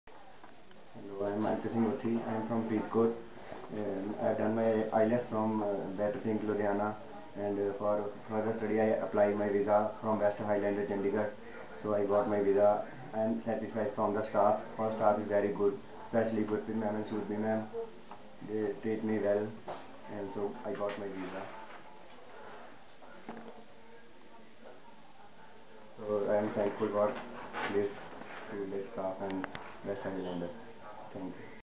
Manpreet Singh Othi got visa approved for studying at Grifith University Australia Jan 2014
Manpreet Singh Othi got visa approved for studying at Grifith University Australia (Jan 2014) through West Highlander Immigration Consultancy Services Pvt. Ltd. Chandigarh.